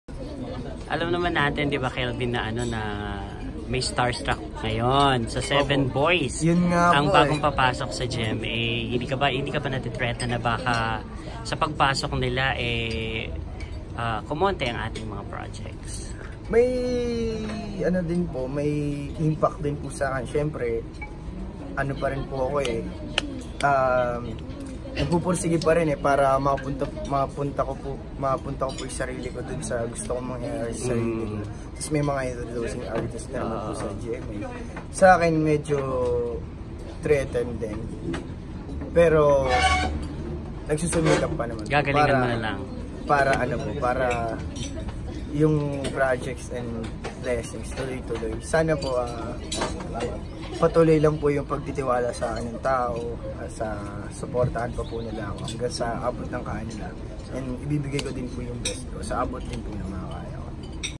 KELVIN MIRANDA NATI-THREATEN SA STARSTRUCK BATCH 7 BOYS ?
KELVIN MIRANDA “ THE FATE “